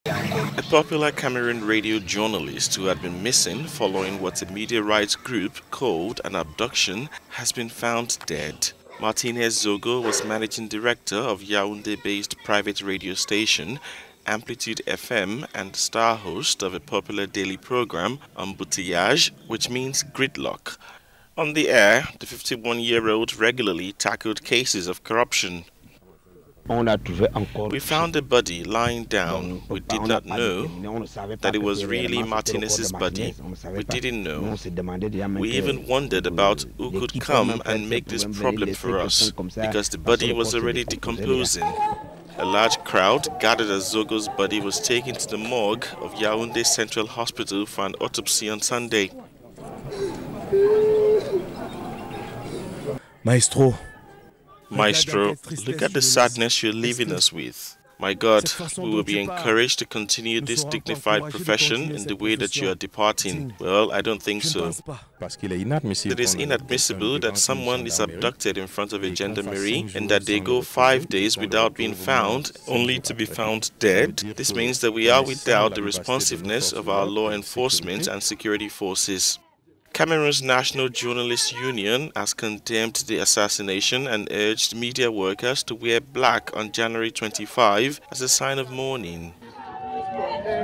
Missing Cameroon journalist Martinez Zogo found dead
"We found a body lying down. We didn't know, we didn't know that it was really Martinez's body, we didn't know. We even wondered 'but who could come and make this problem for us, to come and leave someone like that' because the body was already decomposing." Isidore Onana, witness, resident of Soa